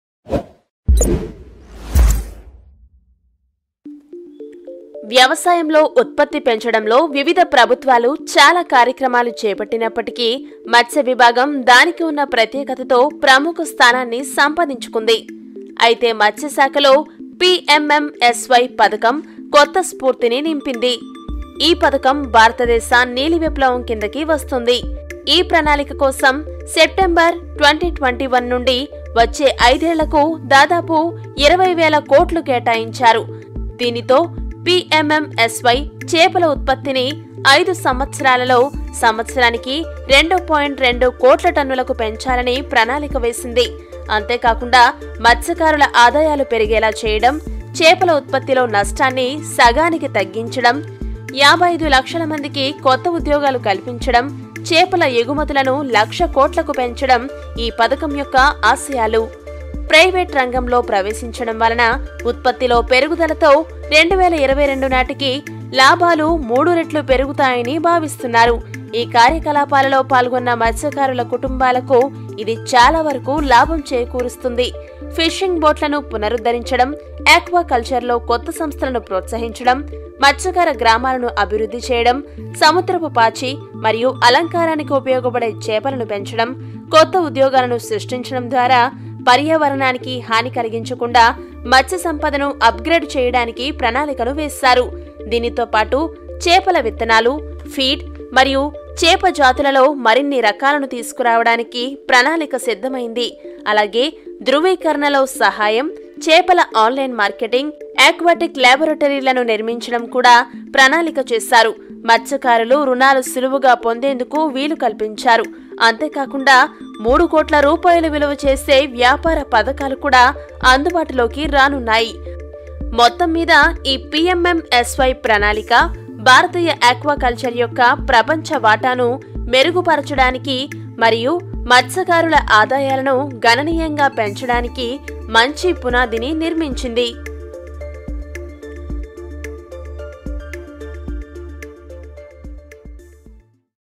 నీలి విప్లవానికి భారత ప్రభుత్వం ఎలా సహకరిస్తోంది | Telugu
వ్యవసాయంలో ఉత్పత్తి పెంచడంలో వివిధ ప్రభుత్వాలు చాలా కార్యక్రమాలు చేపట్టినప్పటికీ, మత్స్య విభాగం దానికి ఉన్న ప్రత్యేకతతో ప్రముఖ స్థానాన్ని సంపాదించుకుంది
అయితే మత్స్యశాఖలో PMMSY పథకం కొత్త స్ఫూర్తిని నింపింది. ఈ పధకం భారతదేశ నీలి విప్లవం కిందకి  వస్తుంది, ఈ ప్రణాళిక కోసం సెప్టెంబరు 2021 నుండి వచ్చే ఐదేళ్లకు దాదాపు రూ. 20,000 కోట్లు కేటాయించారు. దీనితో, PMMSY చేపల ఉత్పత్తిని 5 సంవత్సరాలలో సంవత్సరానికి 2.2 కోట్ల టన్నులకు పెంచాలని ప్రణాళిక వేసింది.  అంతే కాకుండా మత్స్యకారుల ఆదాయాలు పెరిగేలా చేయడం, చేపల ఉత్పత్తిలో నష్టాన్ని సగానికి తగ్గించడం, 55 లక్షల మందికి కొత్త ఉద్యోగాలు కల్పించడం ,, చేపల ఎగుమతులను లక్ష కోట్లకు పెంచడం ఈ పథకం యొక్క ఆశయాలు . ప్రైవేట్ రంగంలో  ప్రవేశించడం వలన ఉత్పత్తి లో పెరుగుదలతో 2022 నాటికి లాభాలు మూడు రెట్లు పెరుగుతాయని భావిస్తున్నారు. ఈ కార్యకలాపాలలో పాల్గొన్న మత్స్యకారుల కుటుంబాలకు ఇది చాలా వరకు లాభం చేకూరుస్తుంది.
ఫిషింగ్ బోట్‌లను పునరుద్ధరించడం, ఆక్వాకల్చర్‌లో కొత్త సంస్థలను ప్రోత్సహించడం, మత్స్యకార గ్రామాలను అభివృద్ధి చేయడం, సముద్రపు పాచి మరియు అలంకారానికి ఉపయోగపడే చేపలను పెంచడం ,, కొత్త ఉద్యోగాలను సృష్టించడం ద్వారా పర్యావరణానికి హాని కలిగించకుండా మత్స్య సంపదను అప్‌గ్రేడ్ చేయడానికి ప్రణాళికను వేశారు. దీనితో పాటు, చేపల విత్తనాలు, ఫీడ్ మరియు చేప జాతులలో మరిన్ని రకాలను తీసుకురావడానికి ప్రణాళిక సిద్దం అయింది, అలాగే ధృవీకరణలో సహాయం, చేపల ఆన్‌లైన్ మార్కెటింగ్, ఆక్వాటిక్ లేబొరేటరీలను నిర్మించడం కూడా ప్రణాళిక చేశారు. మత్స్యకారులు రుణాలు సులువుగా పొందేందుకు వీలు కల్పించారు అంతేకాకుండా 3కోట్ల రూపాయలు విలువ చేసే వ్యాపార పధకాలు కూడా అందుబాటులోకి రానున్నాయి
మొత్తంమీద, ఈ PMSSY ప్రణాళిక భారతీయ ఆక్వాకల్చర్ యొక్క ప్రపంచ వాటాను మెరుగుపరచడానికి మరియు మత్స్యకారుల ఆదాయాలను గణనీయంగా పెంచడానికి మంచి పునాదిని నిర్మించింది.